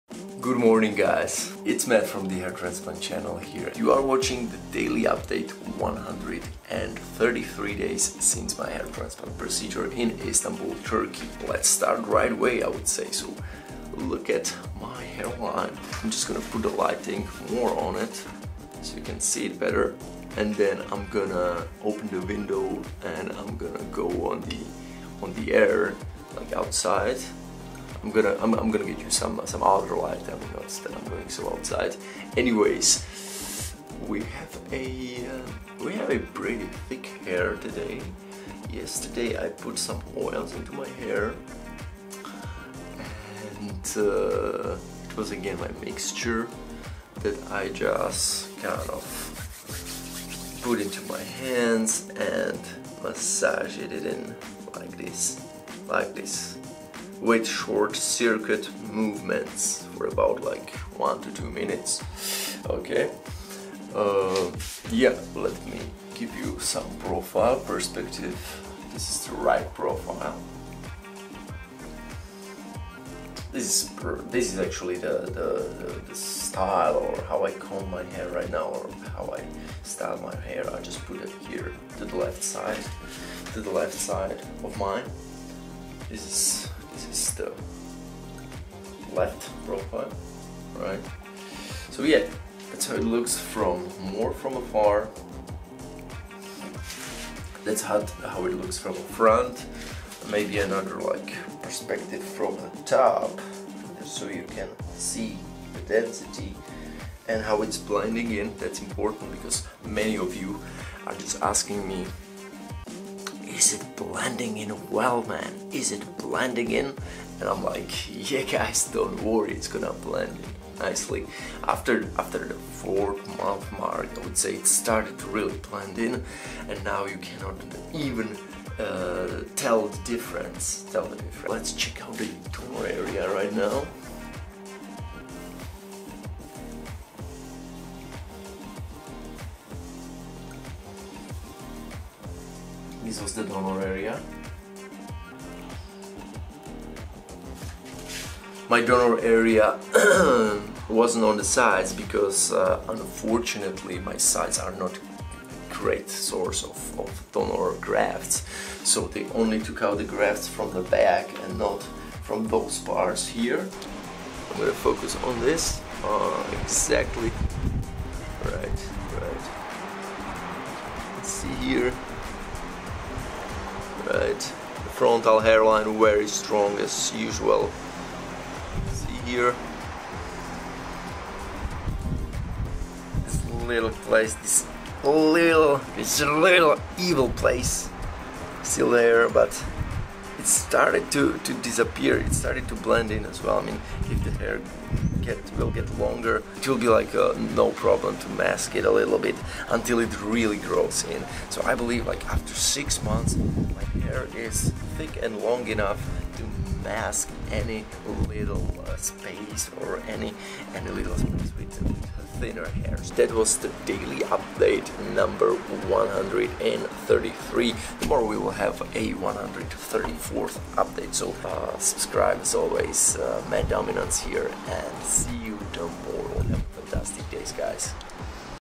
FUE Hair Transplant 133 Days (post op) Istanbul, Turkey GROWTH STAGE
▶️5 Things I wished I had known before my hair transplant (FREE PDF)

133 days/ over 18 weeks after my hair transplant in Istanbul, Turkey.  In this video I am going to show you how my new hairline and donor area are healing and how my transplanted hair starts to grow finally after my shedding phase. Hair growth starts to become more and more evident day by day! Growth stage!

Hair transplant 3500 grafts
FUE hair transplant 3500 grafts